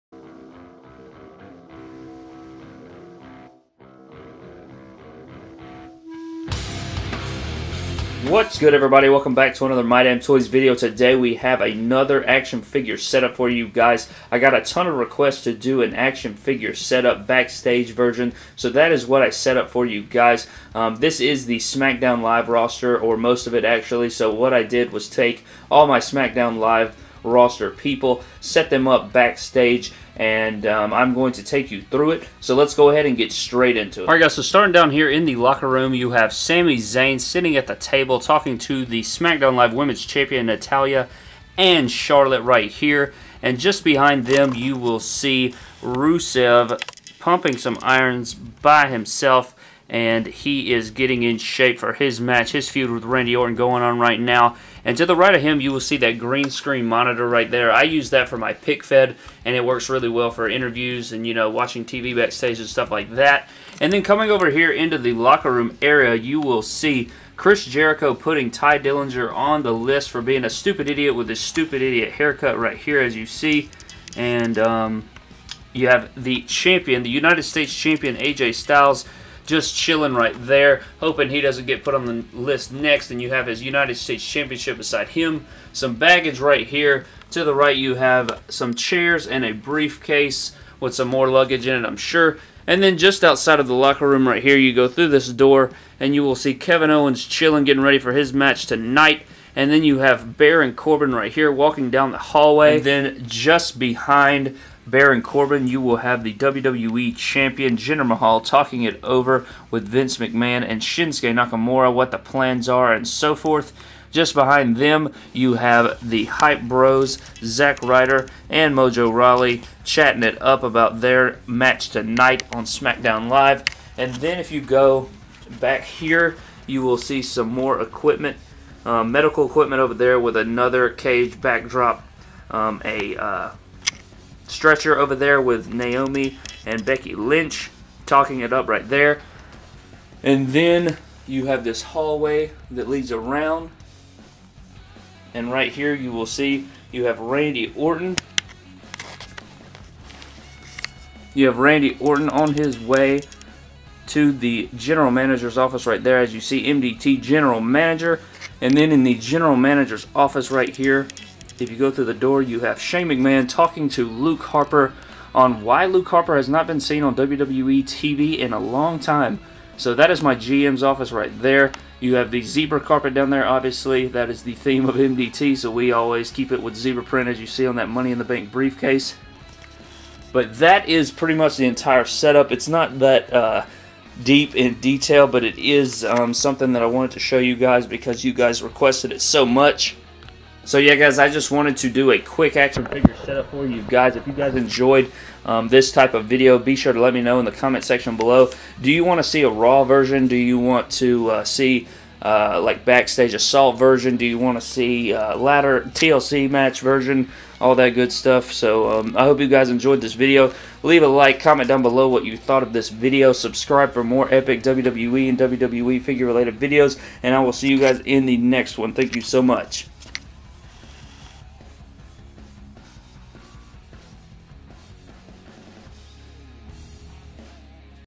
WWE ACTION FIGURE SMACKDOWN LIVE BACKSTAGE SETUP!
In todays video, I will be going through my backstage area set up like Smackdown Live! This video was highly requested from a lot of you so I hope you guys enjoy it!